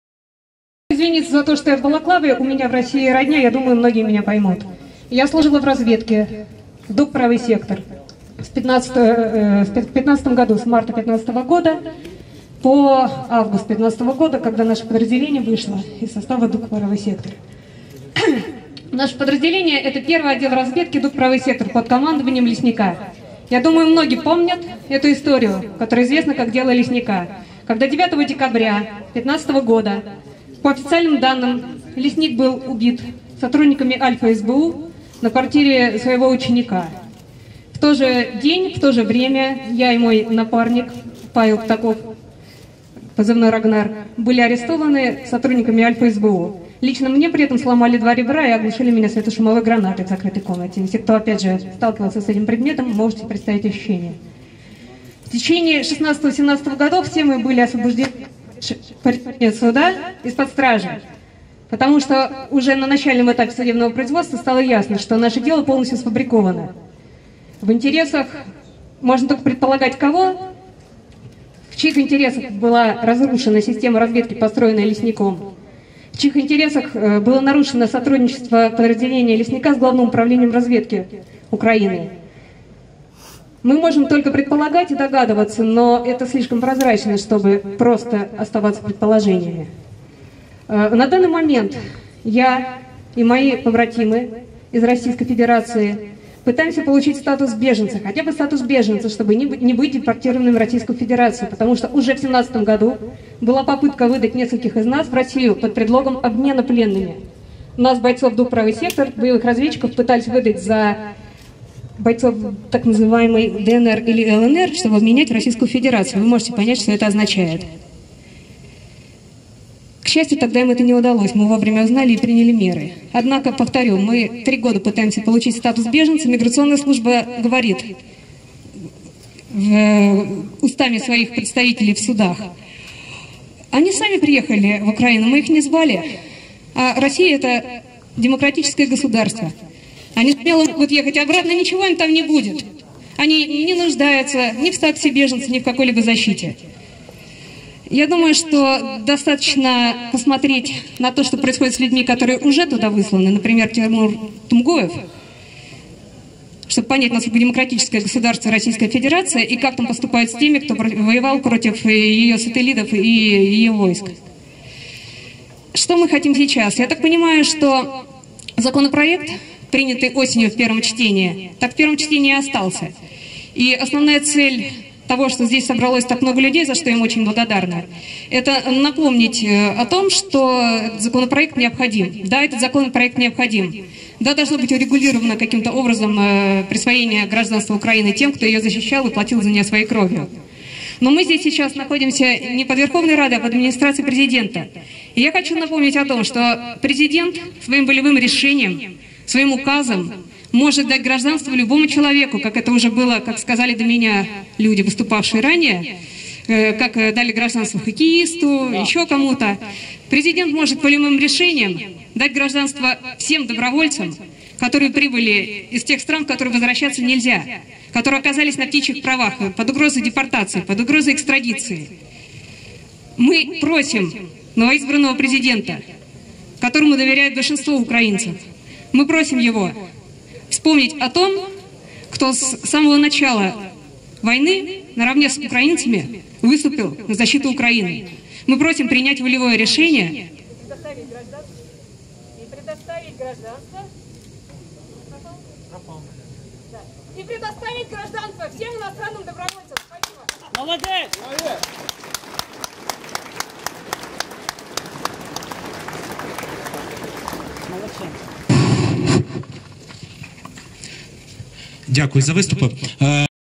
Жінка Доброволець Правий Сектор команда "Лісника" на акції Зе? "Паспорт Добровольцю"!
Активісти "Національного корпусу" і представники ветеранських організацій у вівторок удень у центрі Києва провели мітинг-пікет "Паспорт добровольцю!" з вимогою до президента України Володимира Зеленського спростити процедуру надання громадянства України добровольцям - громадянам іноземних держав, котрі воюють на Донбасі на українському боці.